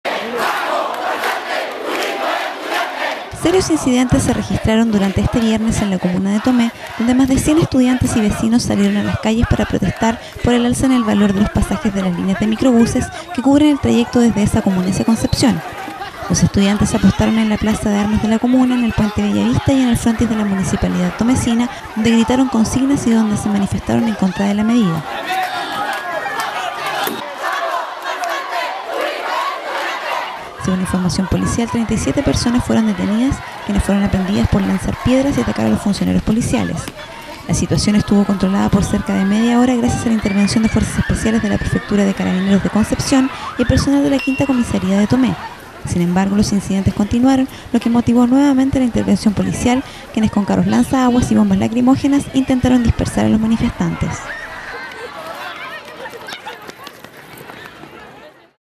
Varios detenidos dejan las protestas de estudiantes en Tomé por el alza en el precio de los pasajes
Serios incidentes se han registrado durante la jornada en la Plaza de Armas y en el frontis de la municipalidad, donde los jóvenes se han enfrentado a Fuerzas Especiales de Carabineros.